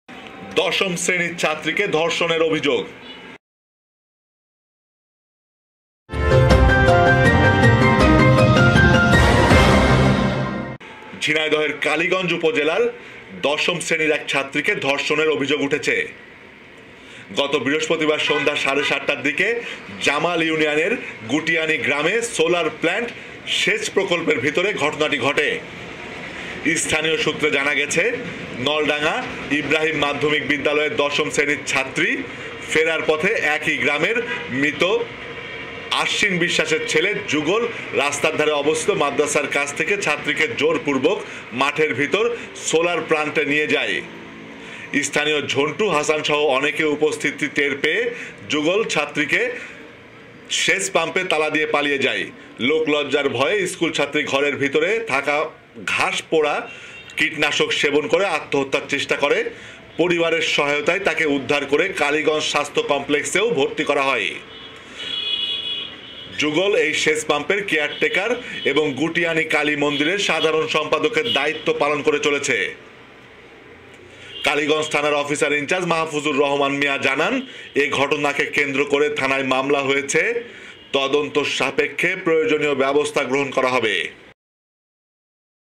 কালীগঞ্জে দশম শ্রেনীর ছাত্রীকে ধর্ষনের অভিযোগ । Kaligonj Student Rape । Jhenaidaher Chokh✔️
ঝিনাইদহের চোখ
ঝিনাইদহ নিউজ